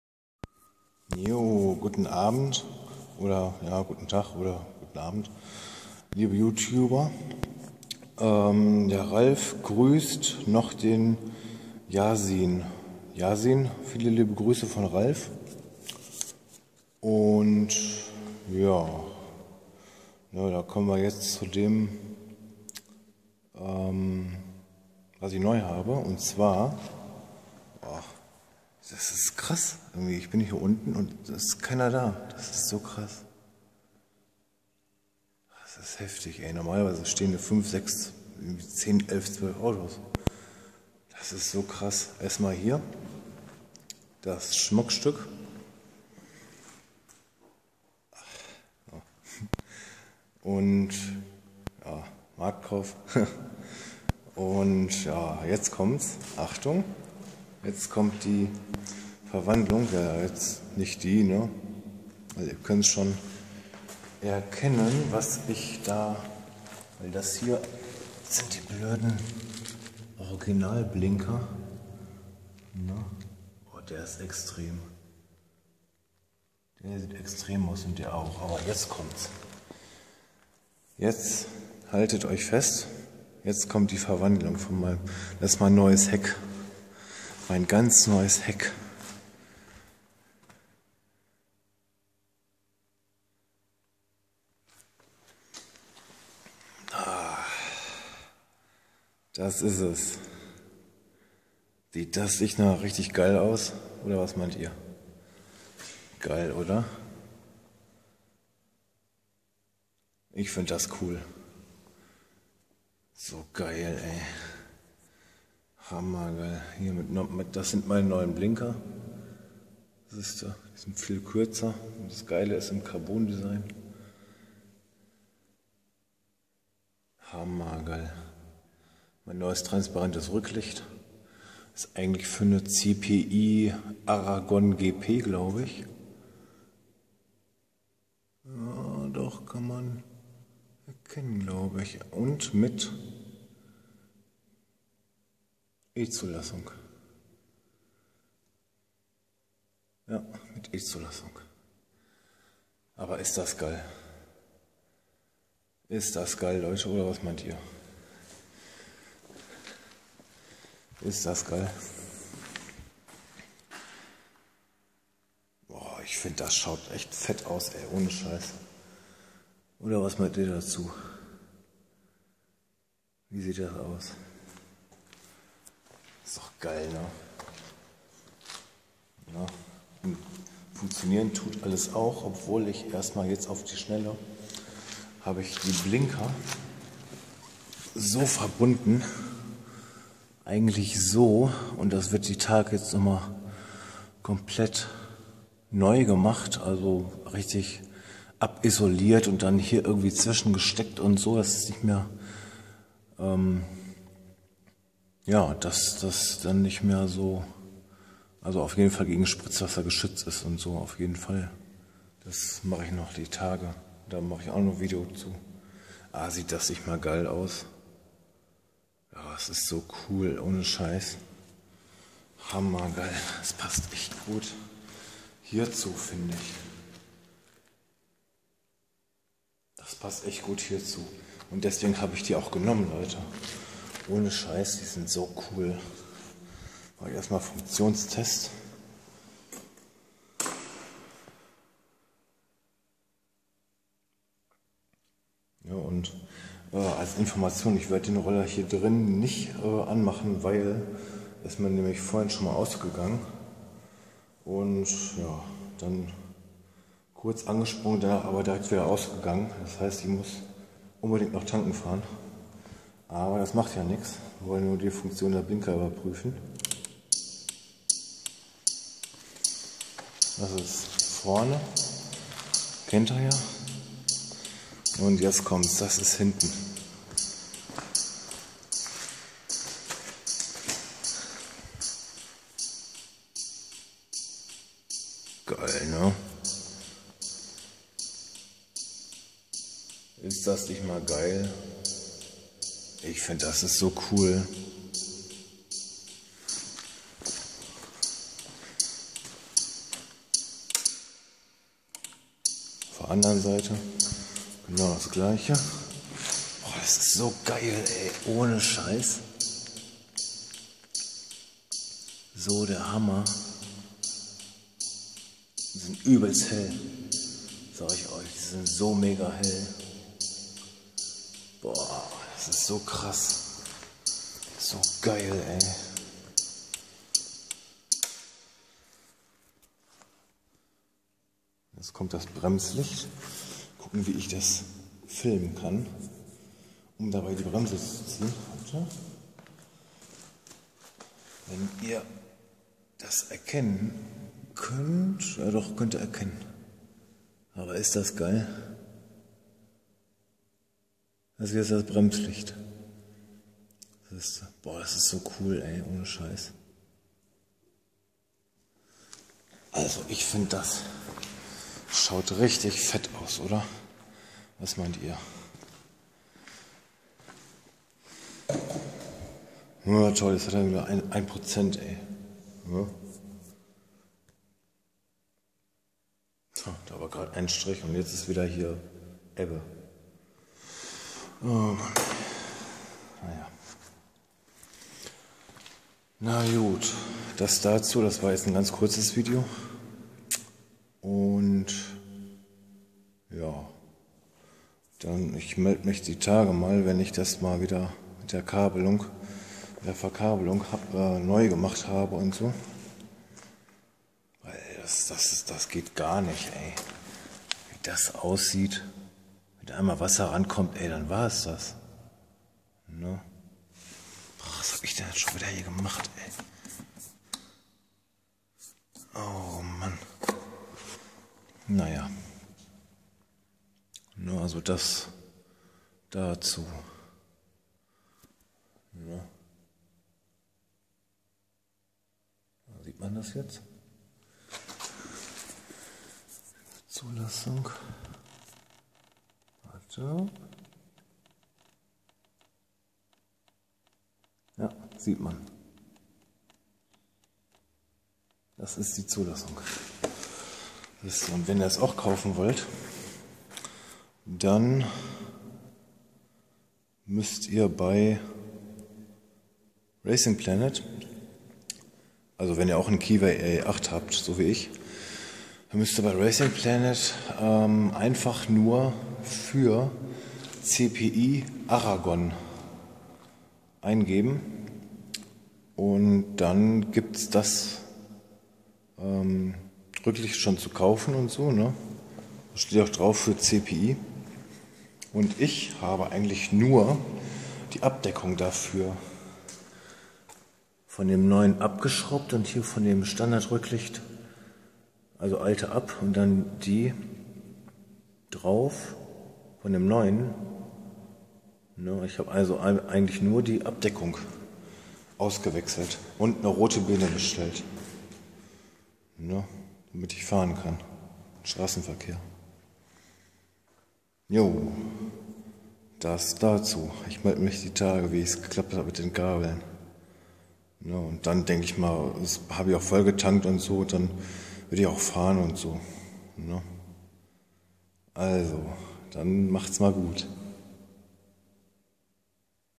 Neue Sachen für mein Keeway ry8 :-)
Wenn ihr das Rücklicht und die Blinker auch haben wollt,
planet.de. Und wenn ihr das auch wie ich haben wollt dann gebt bei racing Planet erst CPI dann ARAGON und dann Elektrick ein und da findet ihr auch die beiden Sachen die ich auch hab ☺